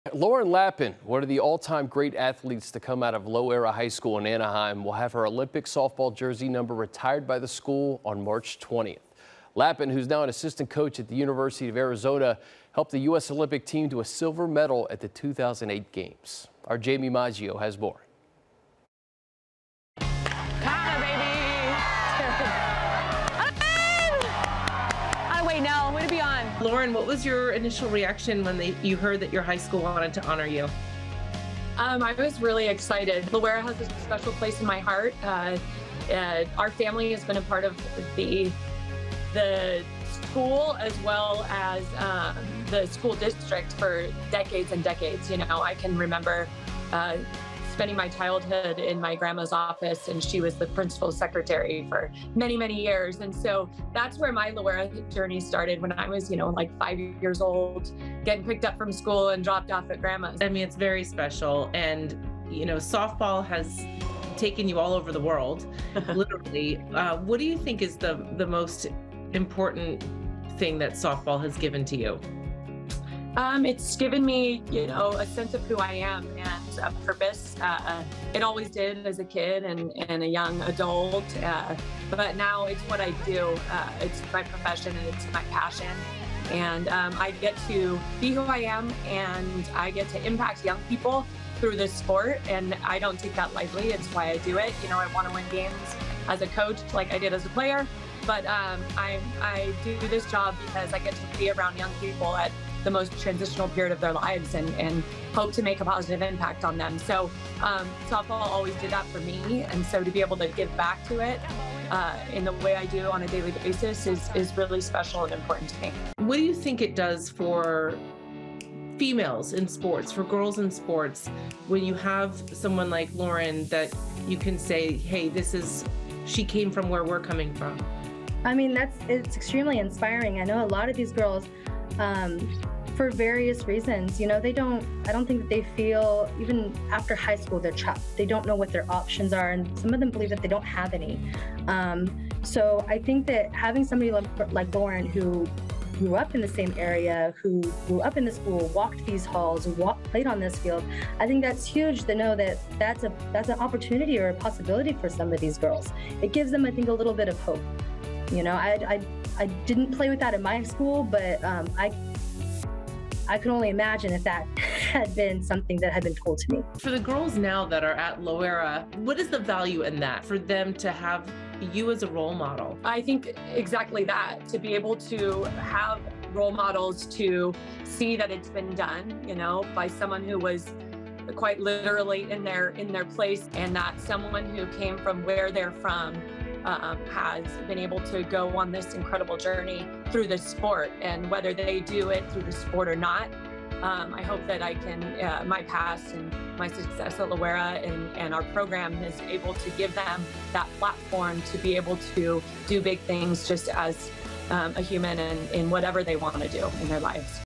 Lappin to be honored at Loara High
USA Softball alum & current Arizona assistant coach Lauren Lappin was a star player at Loara High School before going on to medal at the Olympics. Now, she'll have her number retired at her alma mater as she continues to pay it forward